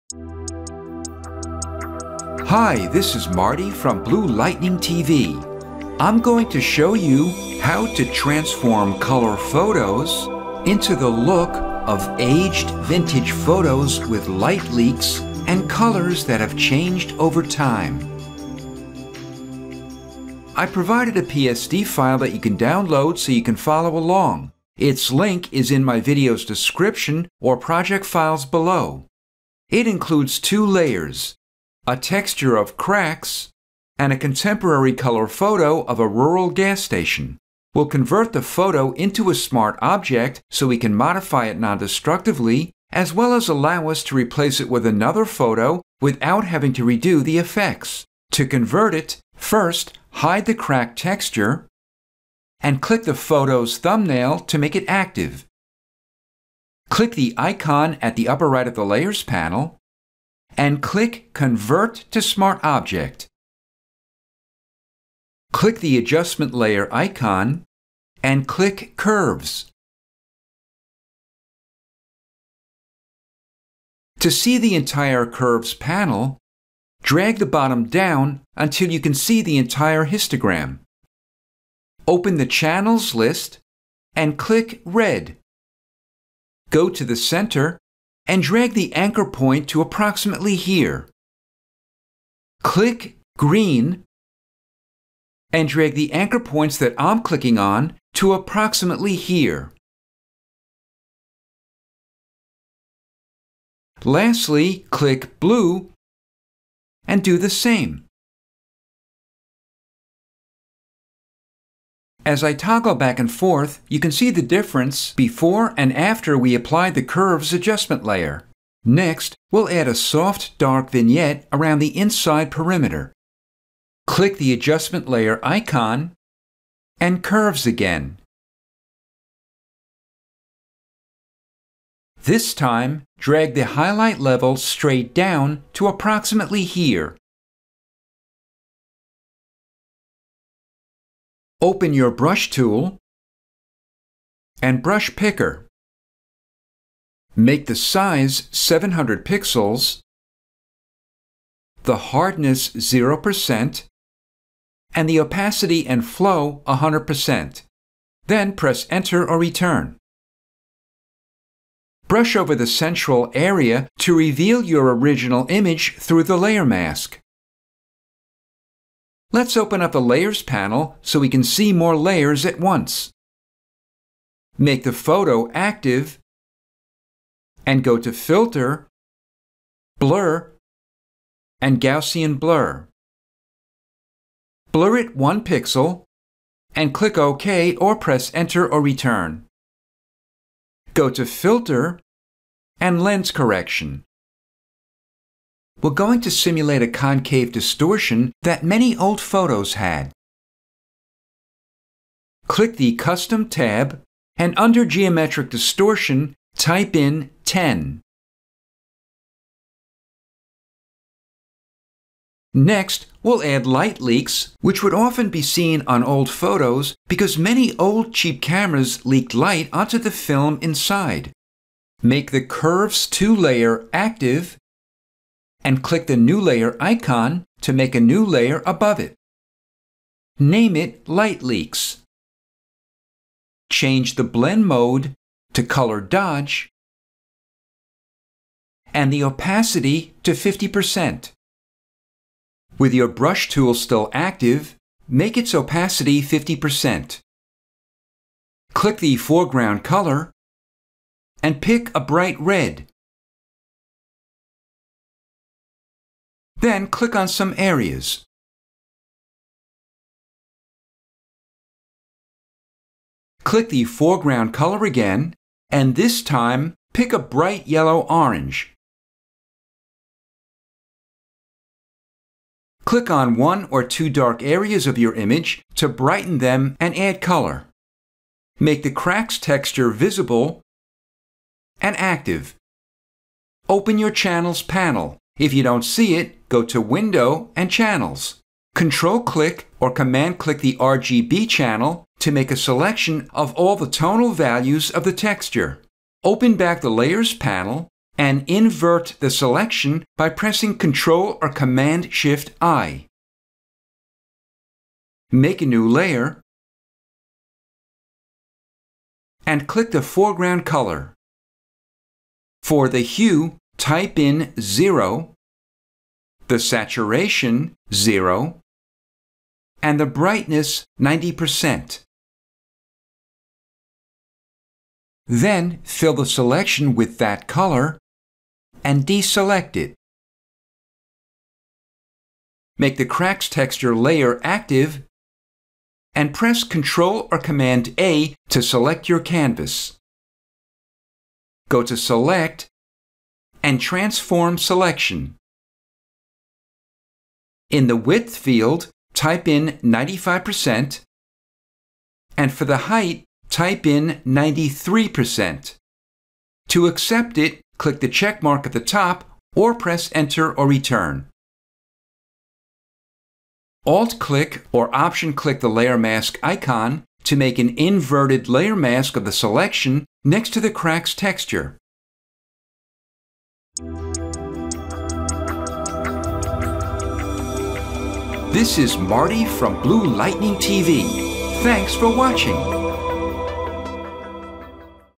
Photoshop: How to Create the Look of Vintage, Color Photos
Photoshop CC 2018 tutorial showing how to transform a contemporary image into the look of an authentic, vintage photo with light leaks and paper cracks.

Give some love back to Blue Lightning TV by becoming my Patron for as little as $2/month: